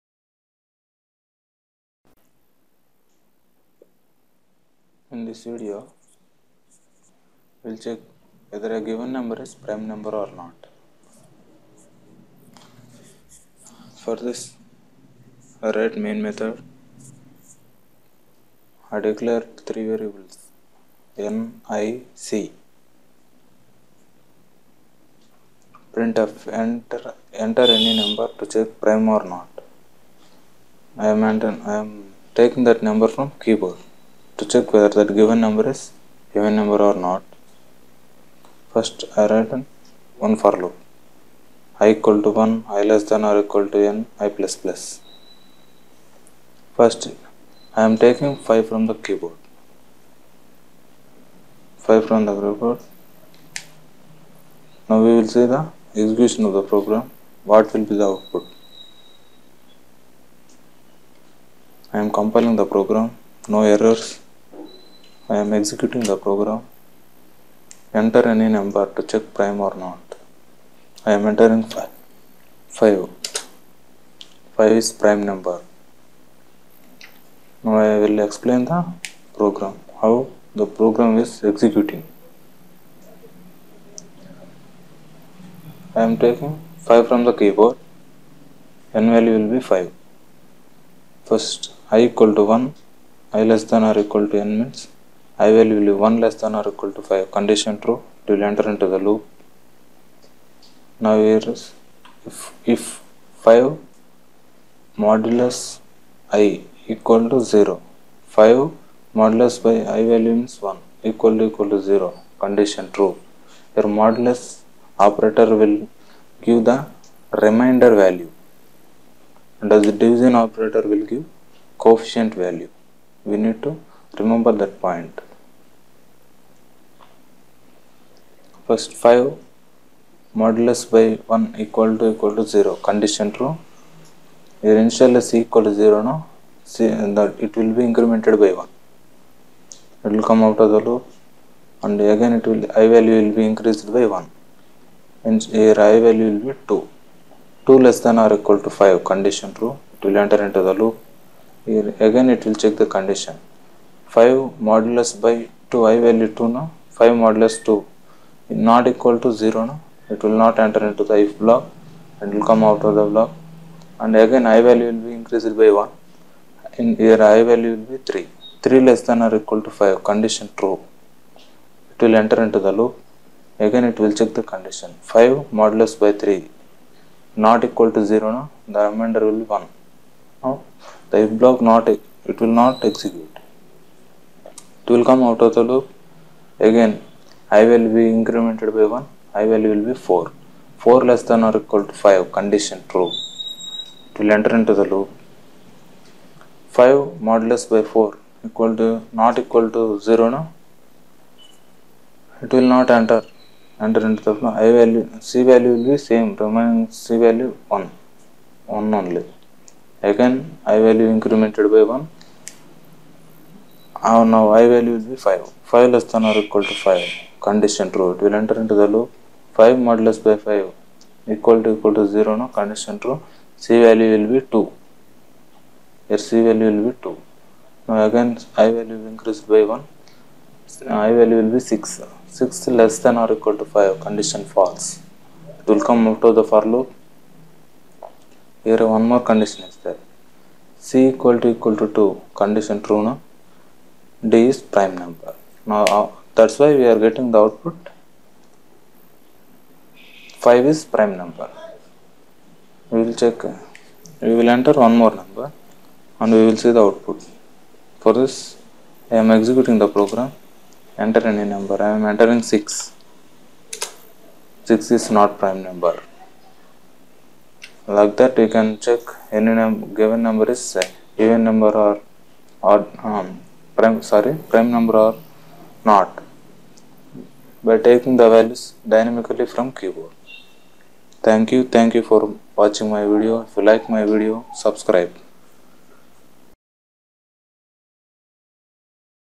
Program to Check Prime Number in C ! Given Number Is Prime Or Not:c
Program to Check Prime Number in C ! Given Number Is Prime Or Not:c.Prime number Program in c with example.c language assignment problems.c language basic problems